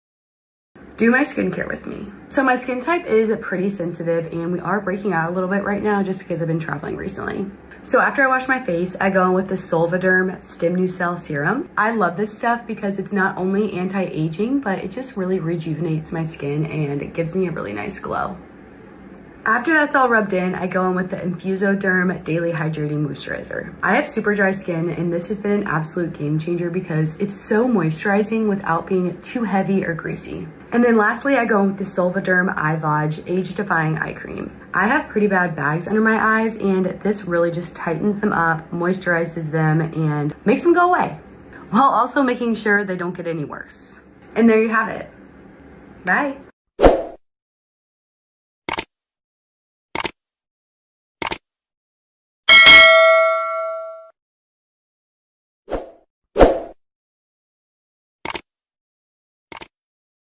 Do My Skincare with Me: Sensitive Skin Routine with Solvaderm | Travel Breakouts & Anti-Aging Faves!
Join me as I walk you through my daily skincare routine for sensitive skin! After some recent travels, my skin has been breaking out, but Solvaderm's products have been lifesavers. Here’s my step-by-step routine:

1. *Stemnucell Serum* – I love how this anti-aging serum rejuvenates my skin, giving it a radiant glow.
2. *Infusoderm Daily Hydrating Moisturizer* – Perfect for my super dry skin, it's a game-changer! Hydrating without being greasy.
3. *Eyevage Age-Defying Eye Cream* – Helps reduce bags and puffiness under my eyes, while preventing further aging.

These products have transformed my skincare, especially with how sensitive my skin is. Check it out and give your skin the glow it deserves!

*Products Mentioned:*
- Stemnucell Serum
- Infusoderm Daily Hydrating Moisturizer
- Eyevage Age-Defying Eye Cream